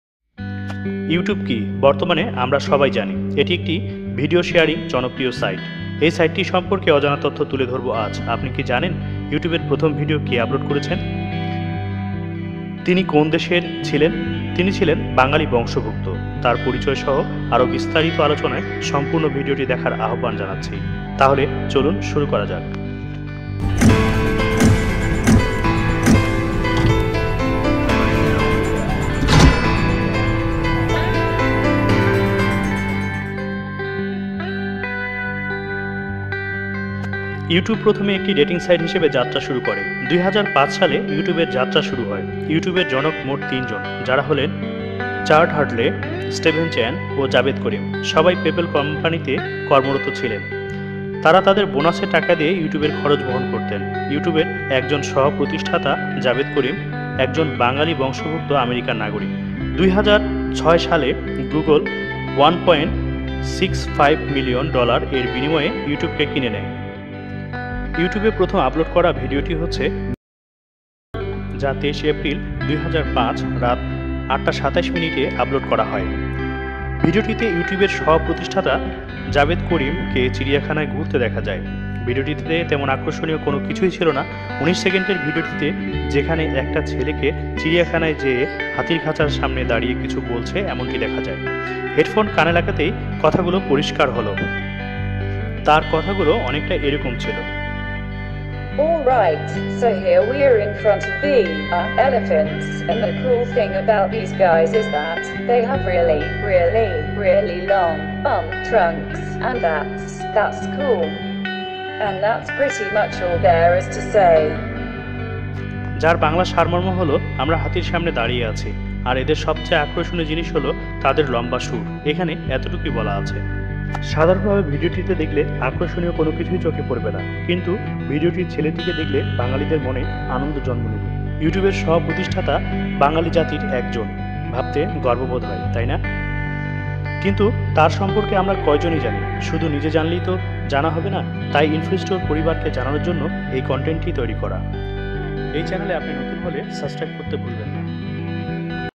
Youtube first uploaded video by a bangli nation (Me at the zoo ) Secret knowadge
আজ আমারা জানবো: Youtube first uploaded video by a bangli nation (Me at the zoo ) Secret knowadge
"Info Store " the best channel.